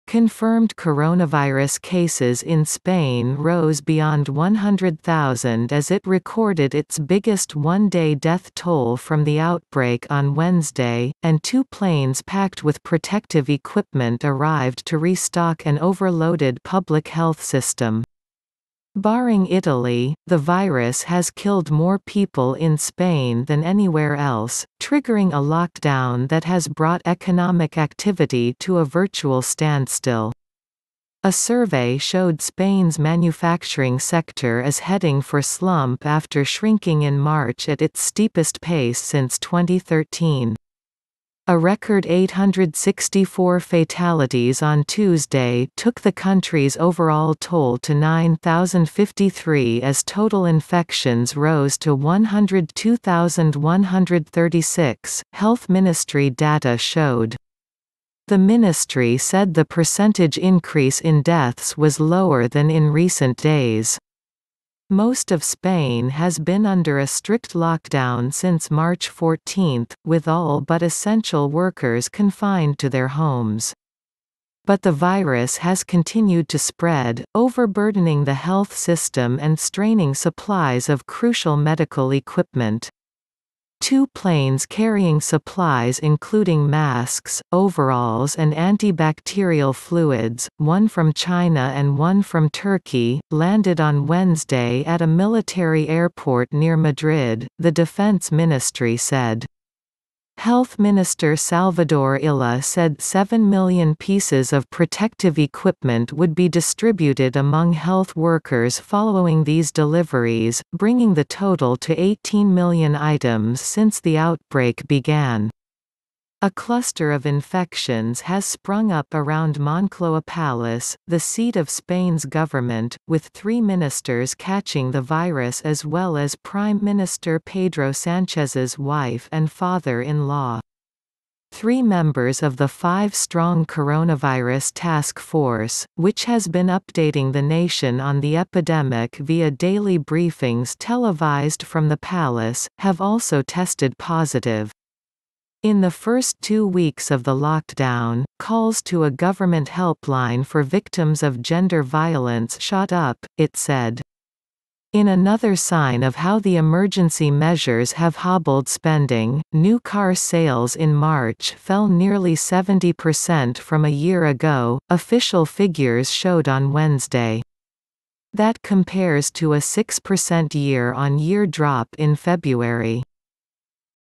Spain's coronavirus cases top 100,000 as masks, sanitizer flown in
Coronavirusupdate Coronavirusoutbreak 👉 Holy Stone HS120D FPV Drone